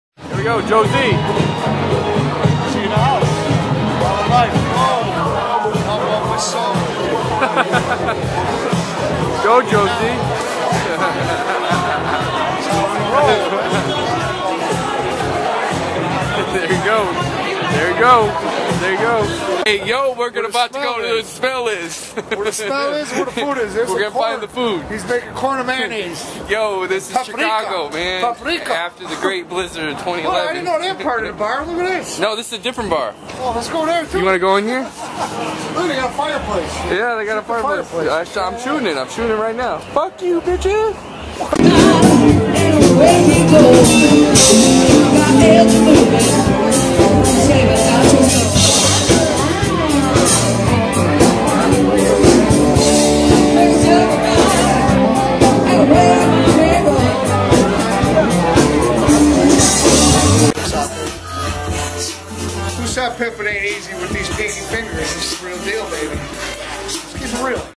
Blues Brothers 2012
Most of the footage was taken from the night of Joe Z & Chief HotFoot @What's In My Headphones... Fundraiser for El Che Rhymefest with some from the night I went to Kingston Mines and there's a sign about no video taping, also I either said fuck you midget, fuck you mitches from a joke at work or i don;t know.